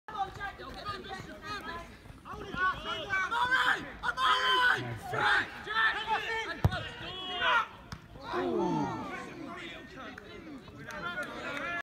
ST v BFC 1H Delgado cross saved
Sawbridgeworth Town v BFC: 1H Omari Delgado's cross is intercepted - video courtesy of Ashley Hanson